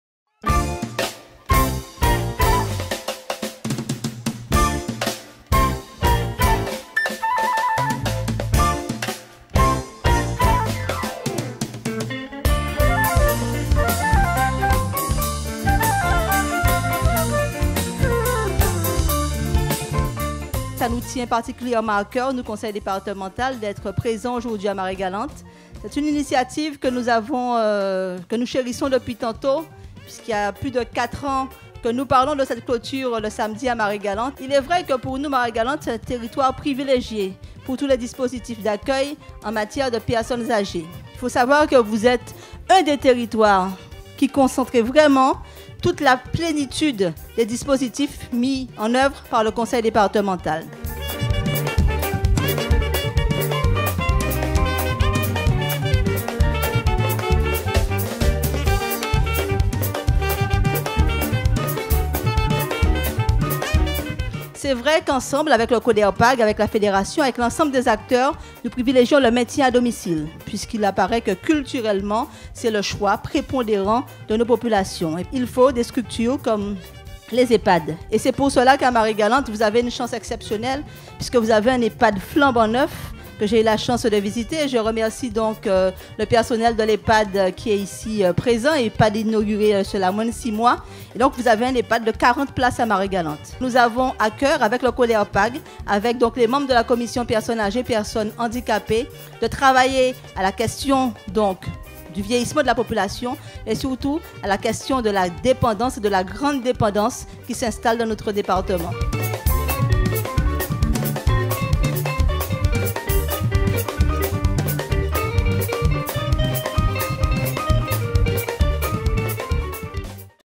SEMAINE BLEUE 2015, Cérémonie de Clôture des iles du sud / Chantal LERUS
SEMAINE BLEUE 2015, Cérémonie de Clôture des iles du sud / Intervention de Mme Chantal LERUS : Président de la Commission des personnes âgées et personnes handicapées du Conseil Départemental, Président délégué du CODERPAG

à l'issue de la Semaine Bleue, semaine nationale des retraités et des personnes agées a eu lieu la Cérémonie de Cloture des Iles du Sud au Chateu Murat à Marie-Galante.

Après une semaine riche en activtés, la collectivité départementale a réaffirmé avec force sa volonté d'accompagner les séniors à bien vivre et bien vieillir quotidiennement.